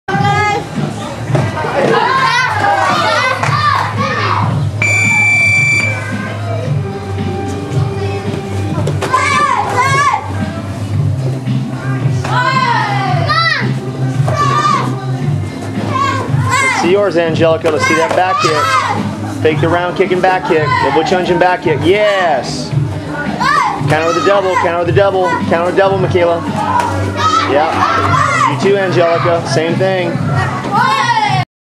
smith Taekwondo kids class
keikis sparring at Smith Taekwondo Center.  Hawaii's largest fulltime martial arts center.